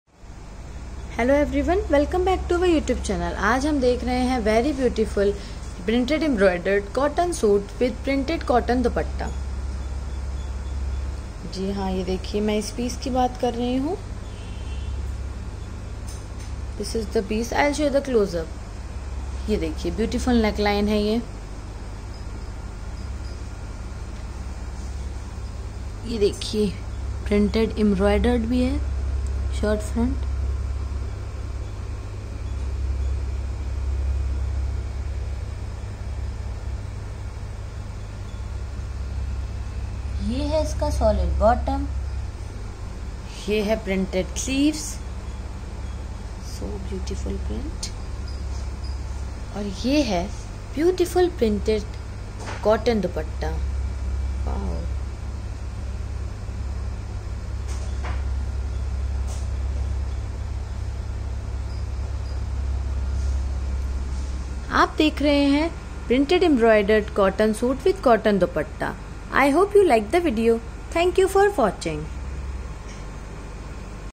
Onion Pink Embroidered Printed Cotton Suit with Printed Cotton Dupatta OR-556557
This is a Live Video of Onion Pink Embroidered Printed Cotton Suit with Printed Cotton Dupatta OR-556557.Exclusively made in House by Fashion From Ornare to Provide Better Understanding of this Beautiful Indian Suit.